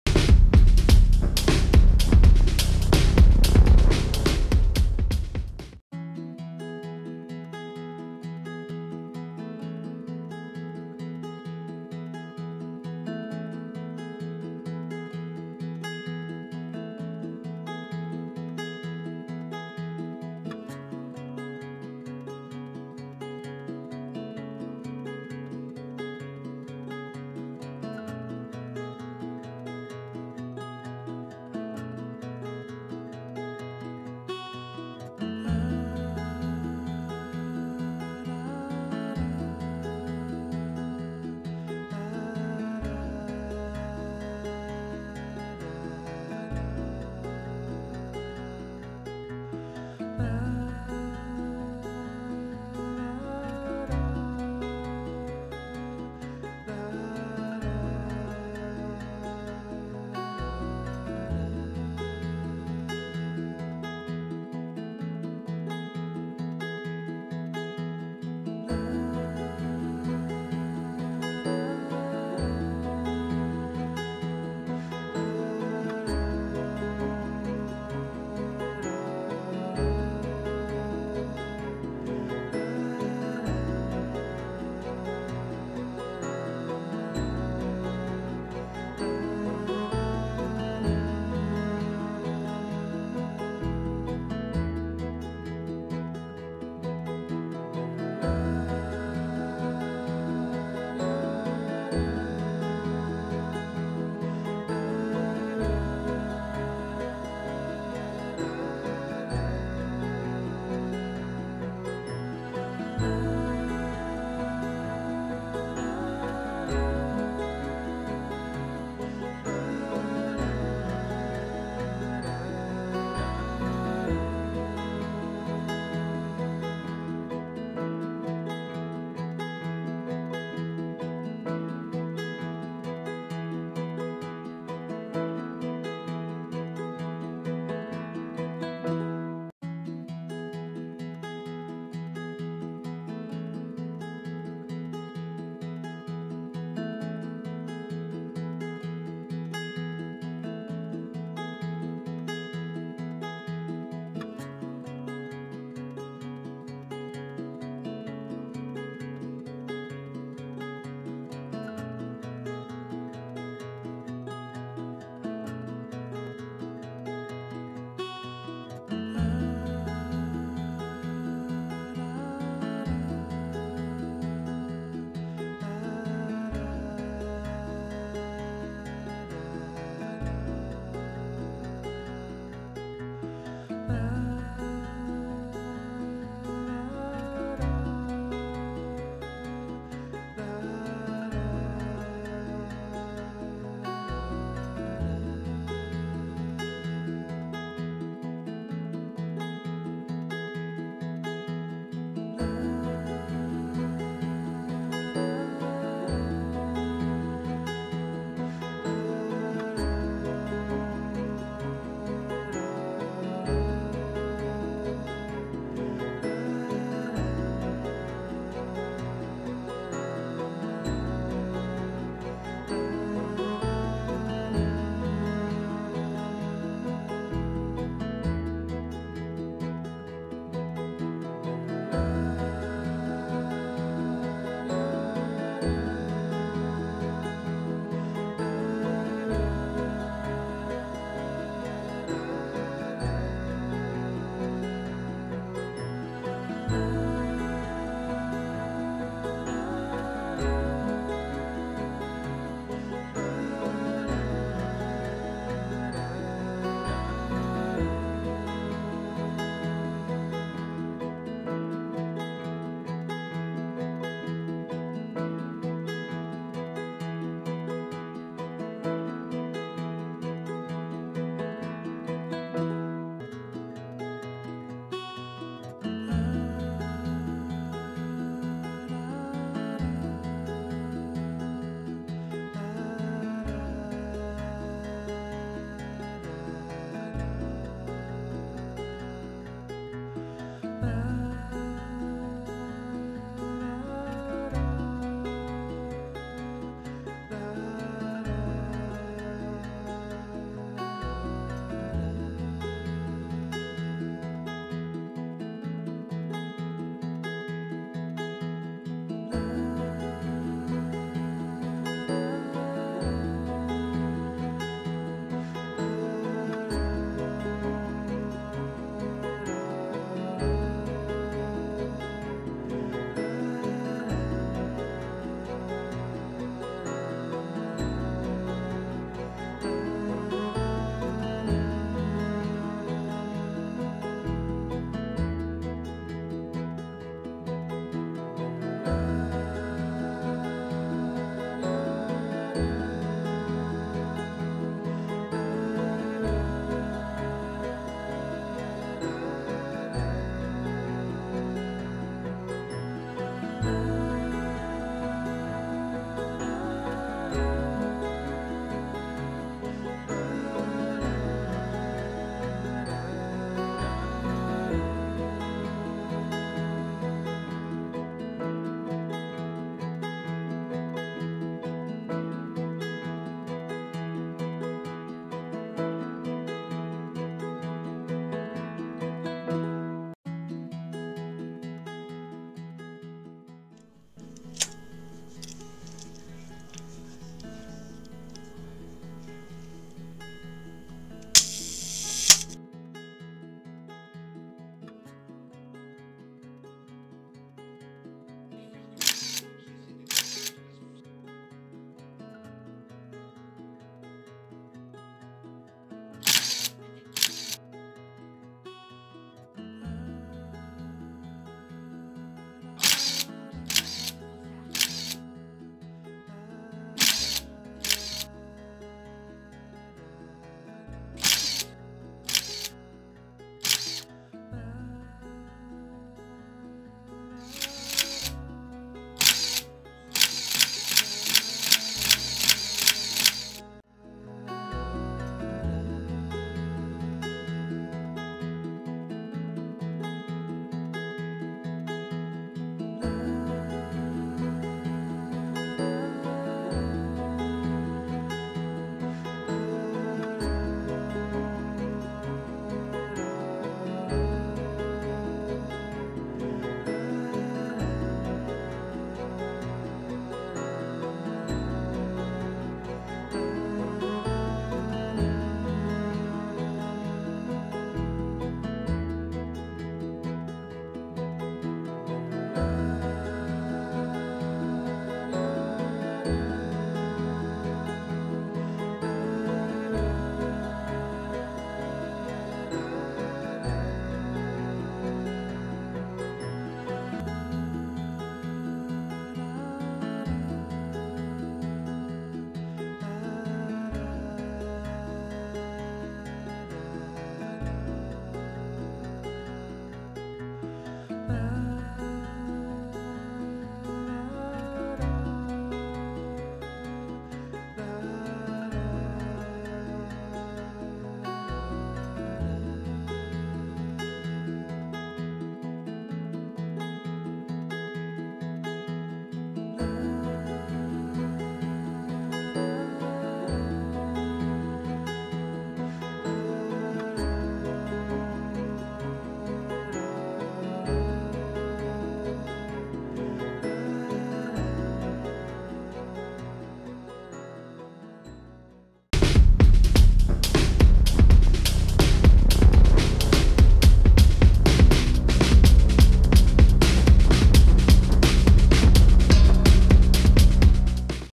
Nikon F pour CLA - Lubrification - partie 2
CLA d'un Nikon F -  Lubrification pas à pas, réglages des rideaux
 et ajustages des vitesses.